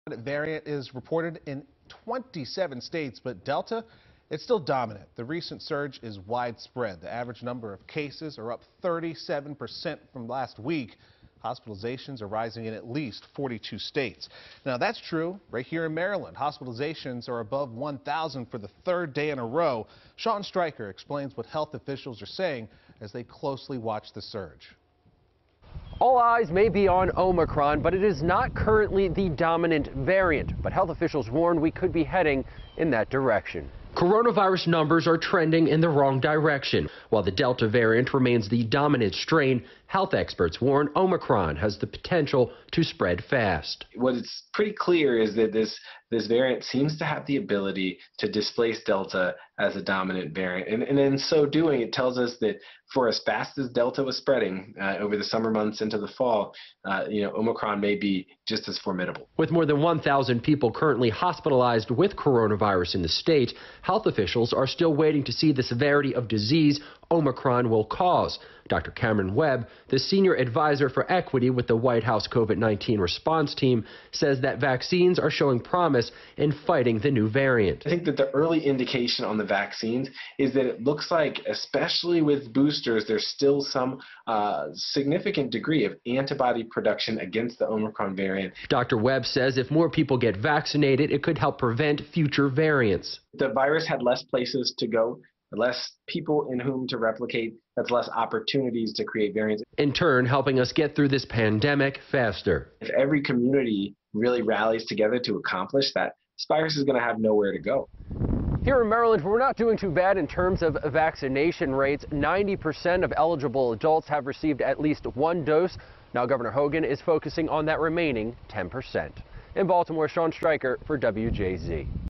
Omicron Has The Potential To Be The Dominant Variant
All eyes are on the omicron variant. While it isn't the dominant variant yet, experts say we could be heading in that direction.